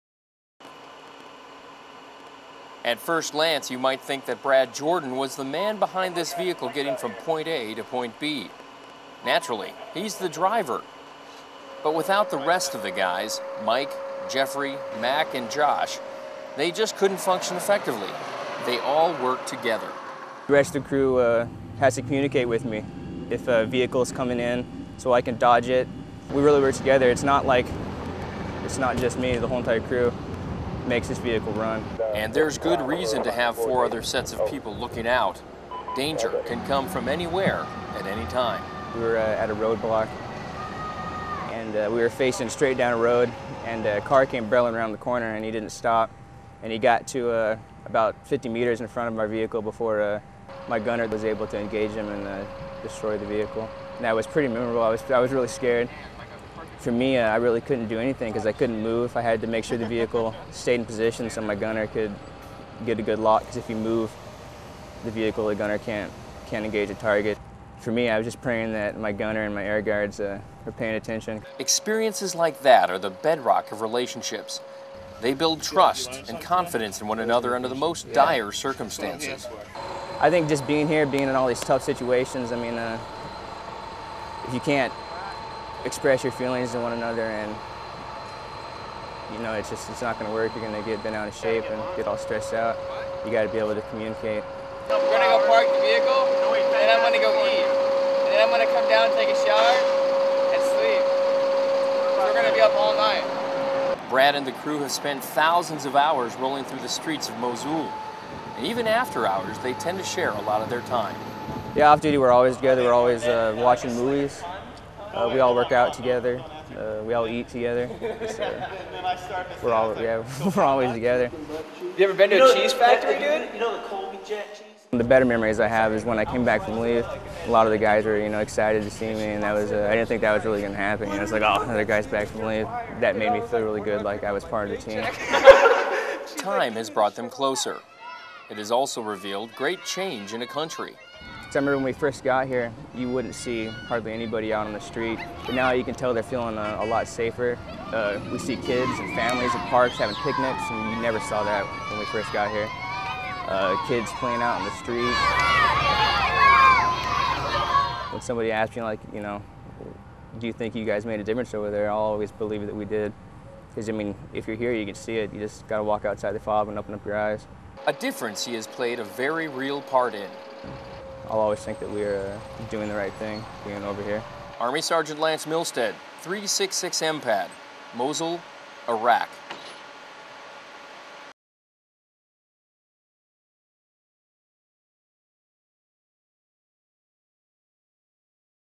Inside a Stryker Team 6-7 Part 1.
This is part one of a five part series about the men that make up the 6-7 Stryker Team with HHC, 3-21 Infantry.